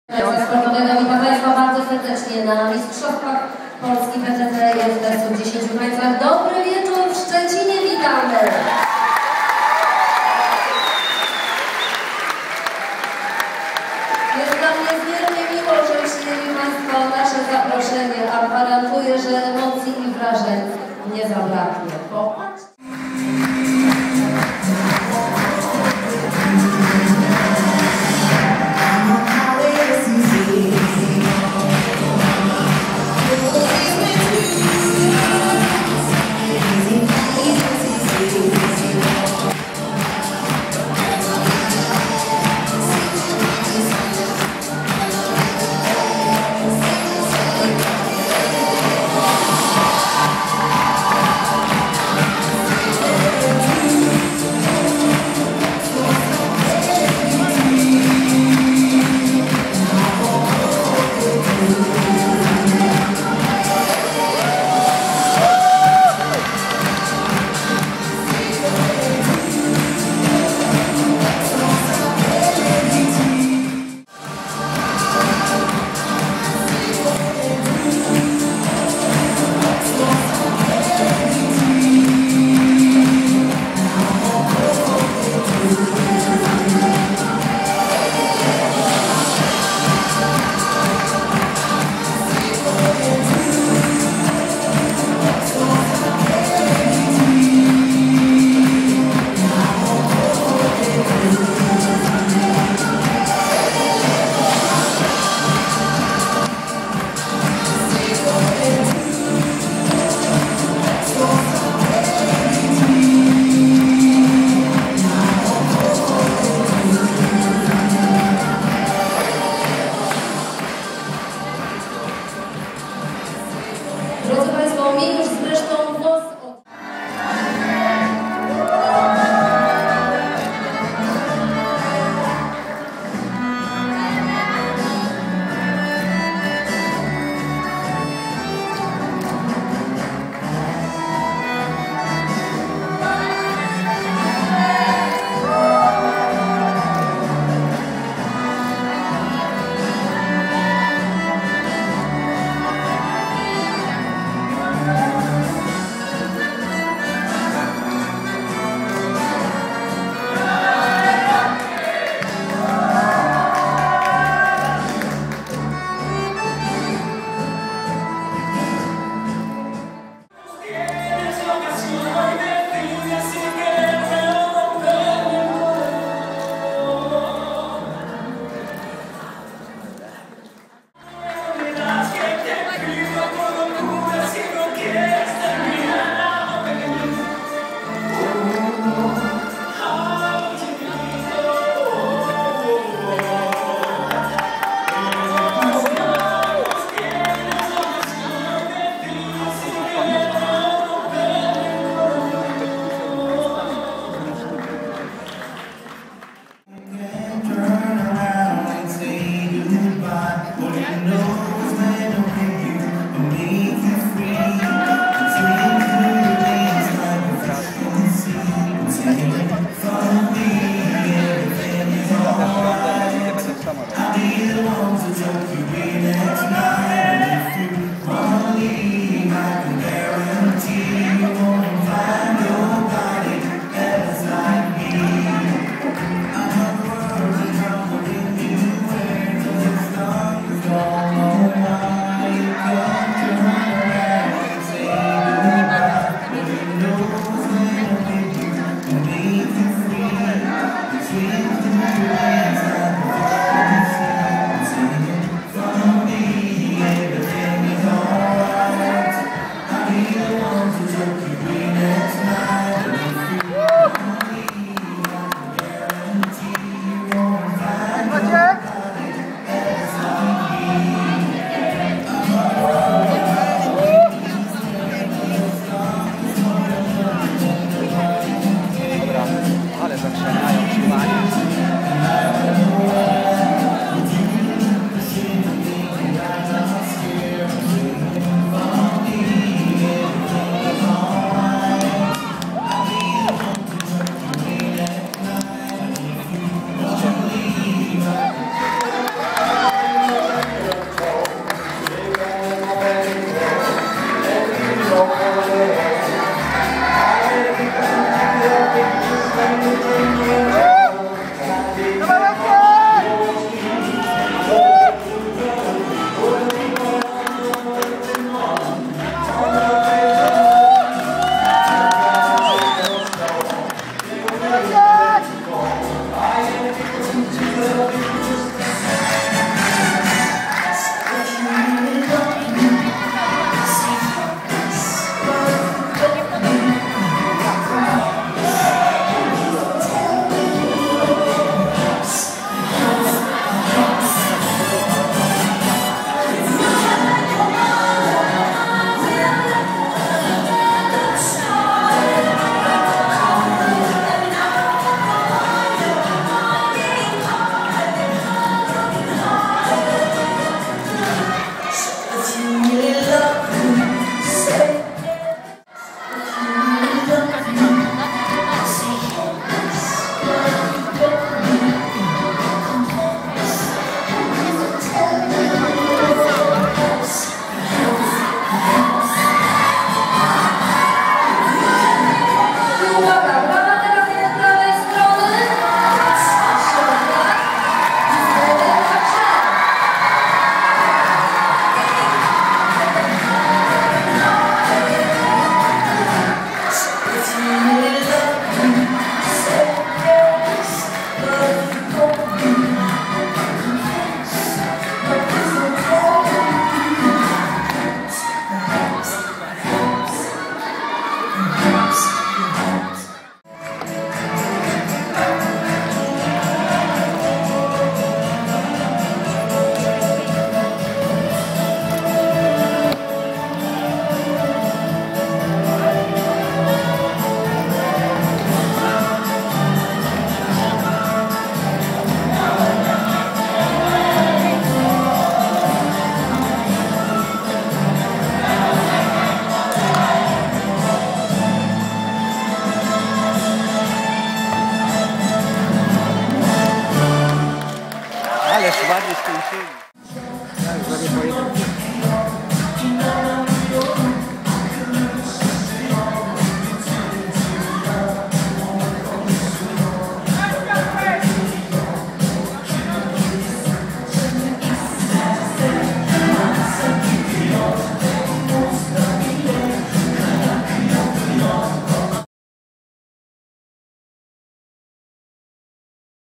Taniec towarzyski w Szczecinie. Autor filmu: Robert Stachnik mov
Szczecin. 13 marca 2011 r. Szczecin był taneczną stolicą Polski. W Szczecińskim Domu Sportu odbyły się mistrzostwa kraju w 10 tańcach. Hala zapełniła się do ostatniego miejsca. Jedną z jurorek jest znana z „Tańca z Gwiazdami" Iwona Pavlovic, a zmaganiom przyglądała się zwyciężczyni ostatniej edycji programu, Monika Pyrek. (kol)
Autor filmu: Robert Stachnik
W finałowej szóstce wśród par dorosłych znaleźli się reprezentanci Szczecina: Marcin Kalitowski i Katarzyna Florczuk (Astra) oraz Tomasz Jóźwiak i Katarzyna Bajdek ( Akademicki Klub Tańca Towarzyskiego).